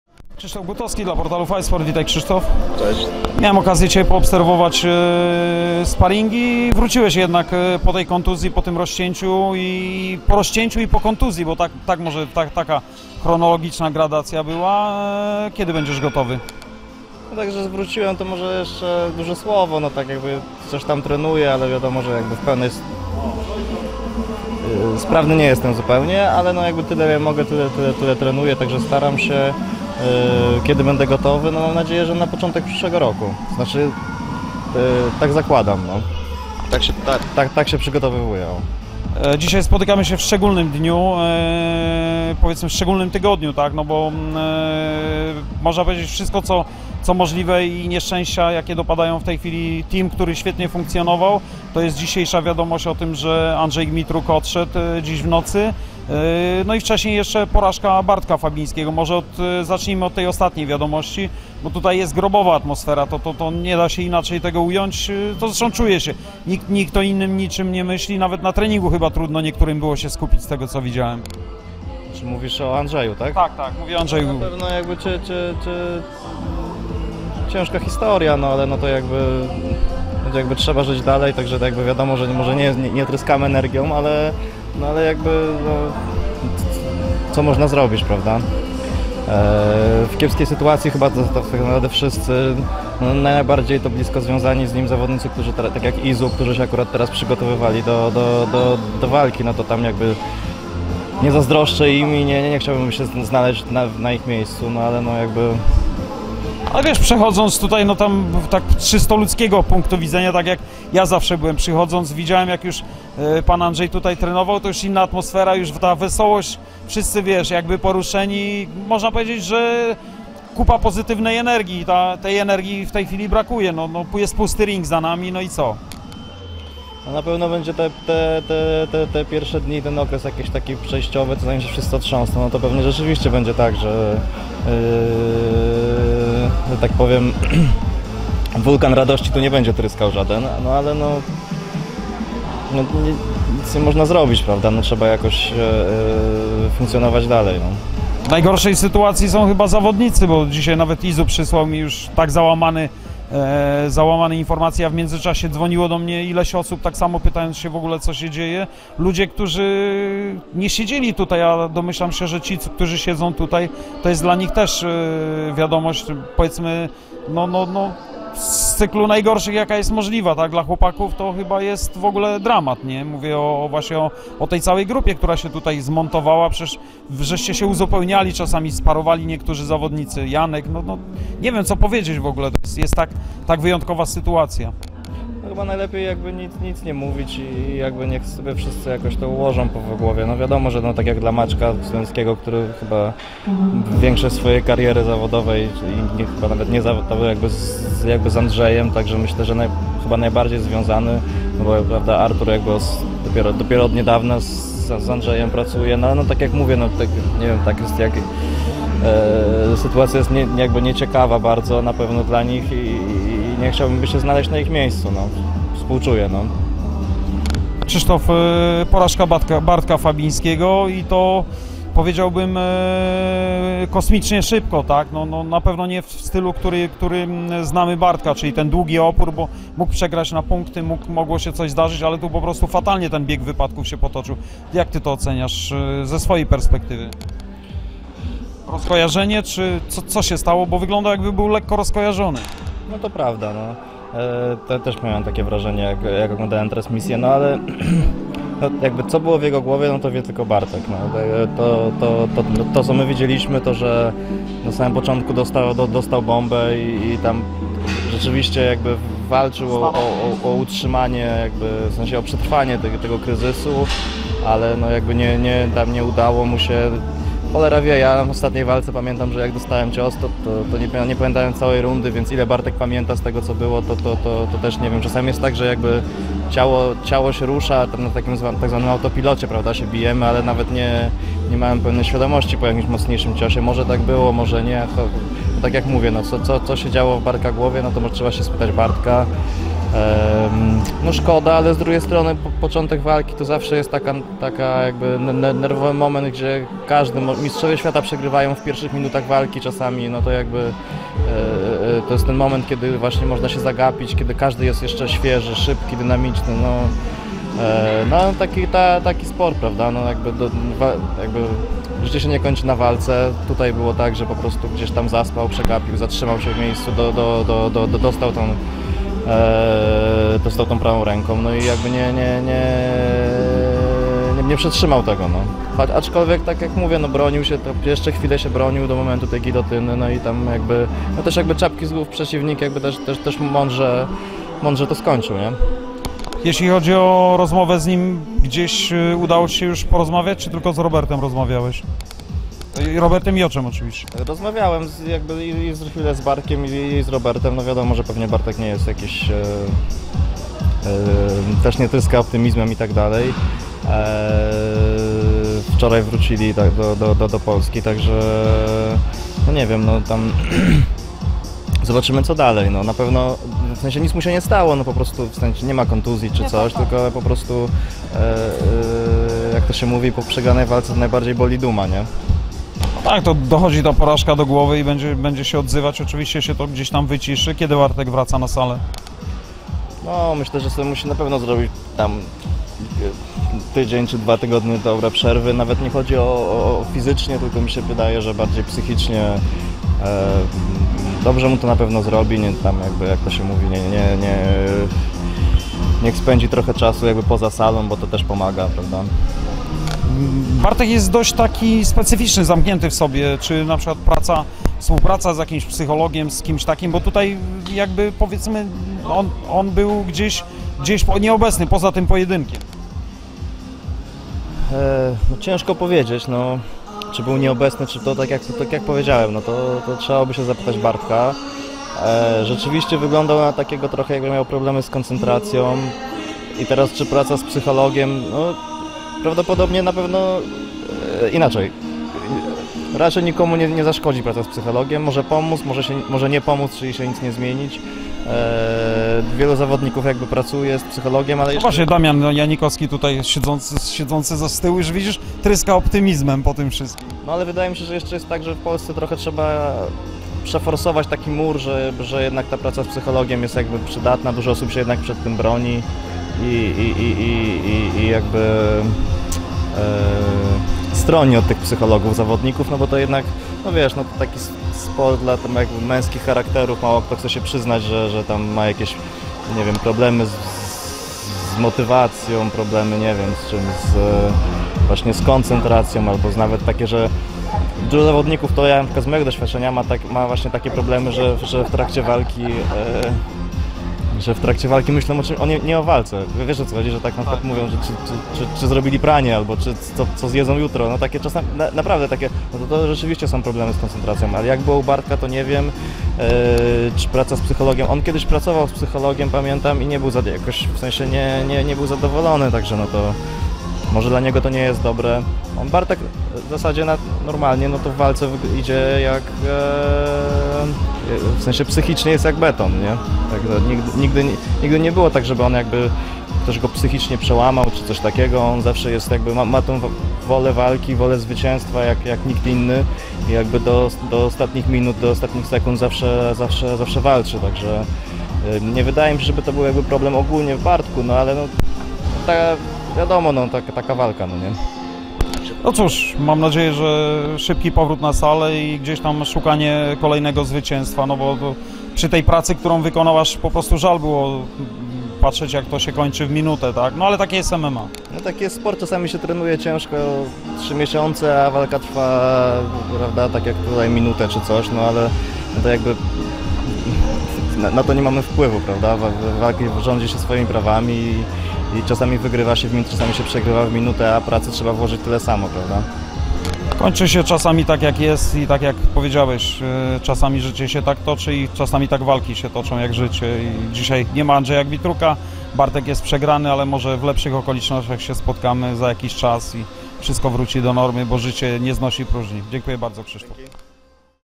Krzysztof Gutowski o Andrzeju Gmitruku i porażce Bartosza Fabińskiego
Trener i zawodnik Berkut WCA Fight Team Krzysztof Gutowski o śmierci Andrzeja Gmitruka i porażce Bartosza  Fabińskiego na UFC Fight Night5 140 Argentina